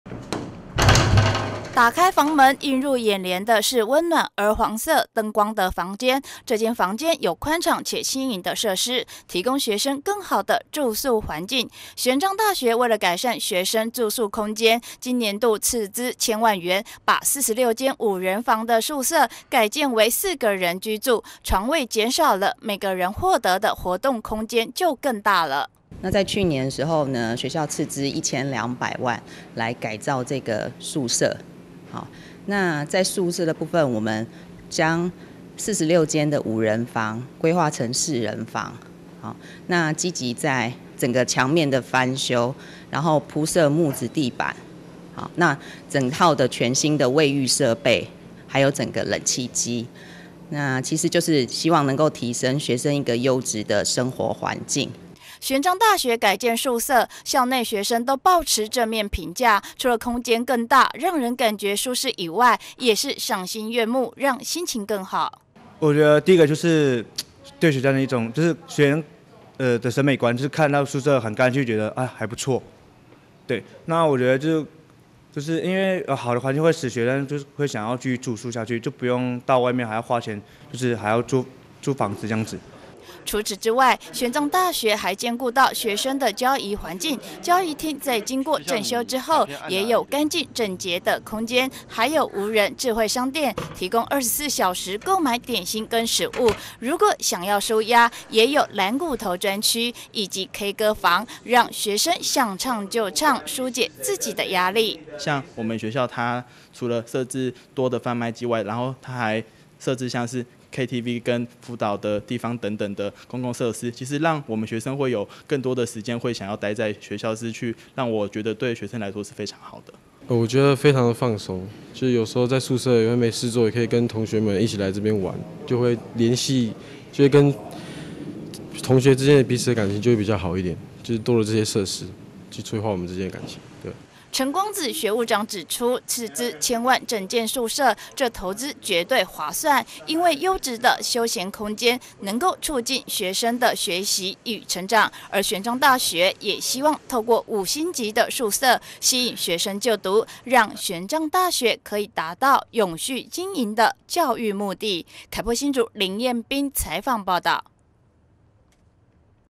20190110N 玄奘大學五星級宿舍 學生住了不想走
為了提供學生更好的宿舍空間，玄奘大學斥資千萬，將校內宿舍大改建，有46間五人房，改為四個人居住，讓宿舍空間更加寬敞舒適，除此之外，玄奘大學也把交誼廳重新整修，加入了無人商店、休憩區跟K歌房，提供學生舒壓空間。